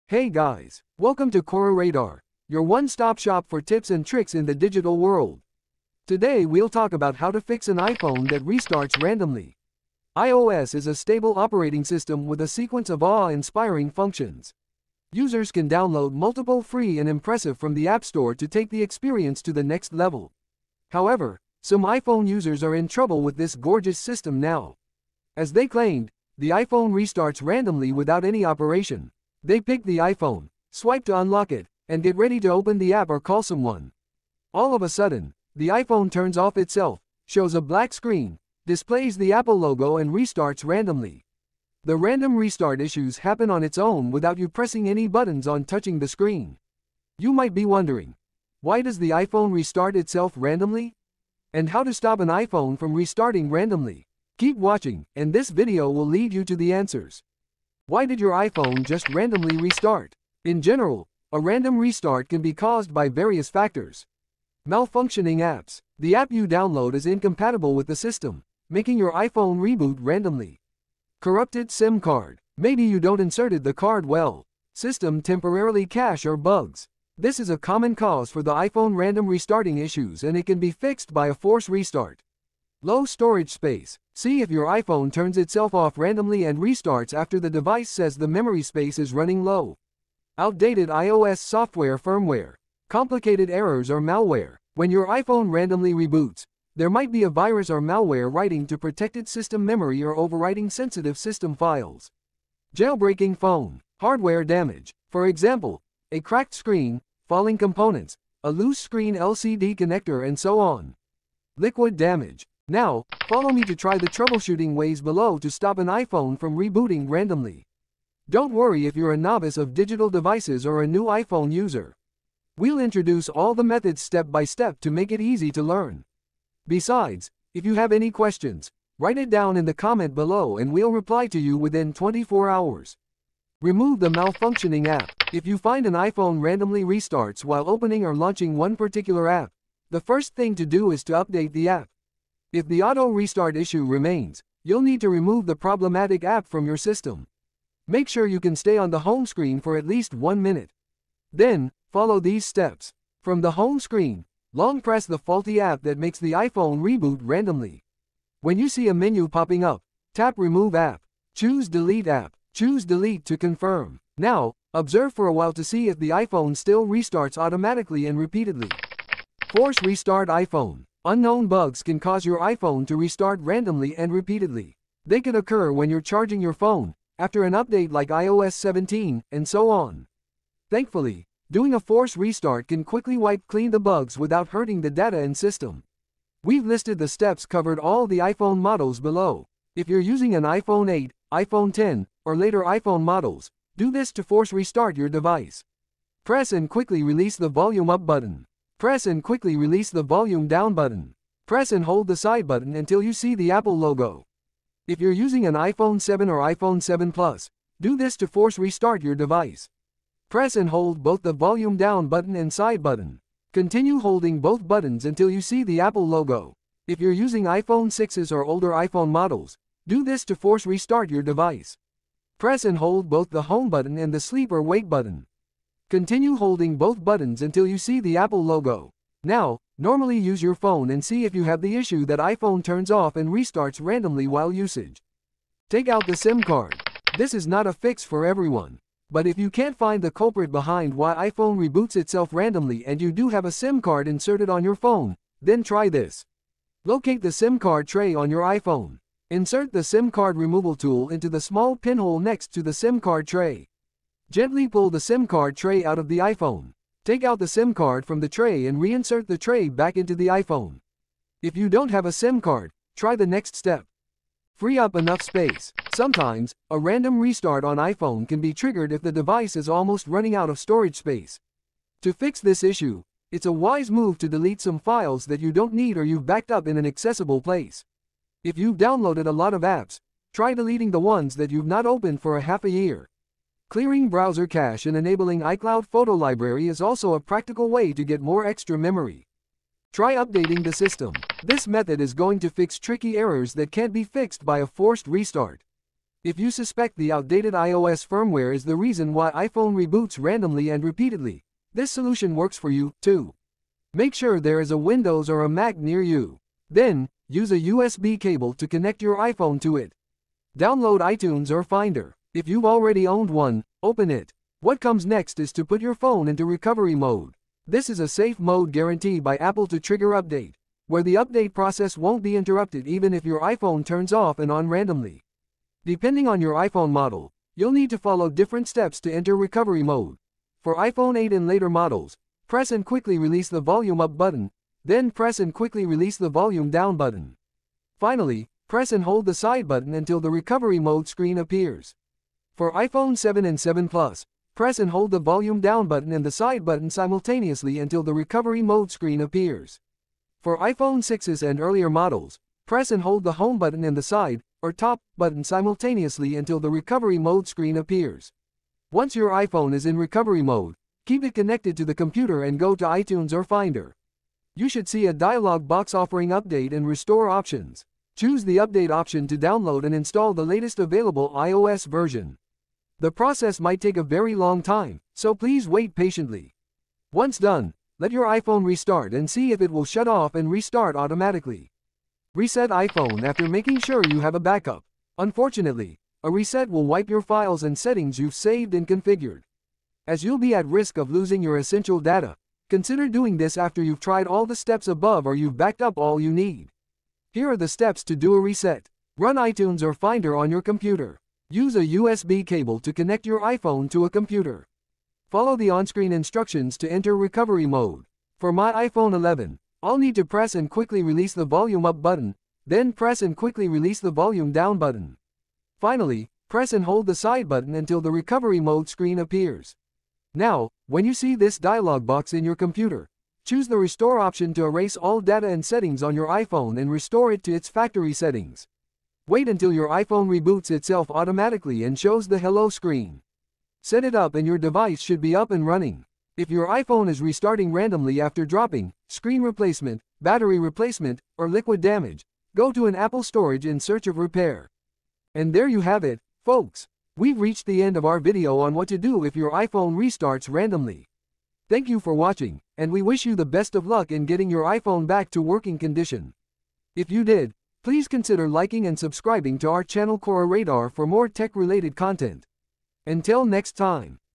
How to Fix an iPhone That Restarts Randomly & Repeatedly (without Warning)
Today we’ll talk about how to fix an iPhone that restarts randomly. Some iPhone users are in trouble with this gorgeous system now. As they claimed, the iPhone restarts randomly without any operation. They pick the iPhone, swipe to unlock it, and get ready to open the app or call someone. All of a sudden, the iPhone turns off itself, shows a black screen, displays the Apple logo and restarts randomly. The random restart issues happen on its own without you pressing any buttons on touching the screen.

You might be wondering: Why does the iPhone restart itself randomly? And how to stop an iPhone from restarting randomly? Keep watching, and this video will lead you to the answers.

0:00 ➡️Opening
0:58 ➡️Why did your iPhone just randomly restart
1:52 ➡️How to fix iPhone that restarts randomly
2:13 ➡️Remove the malfunctioning app
2:49 ➡️Force restart iPhone
3:55 ➡️Take out the SIM card
4:24 ➡️Free up enough space
4:49 ➡️Try updating the system
6:22 ➡️Reset iPhone after making sure you have a backup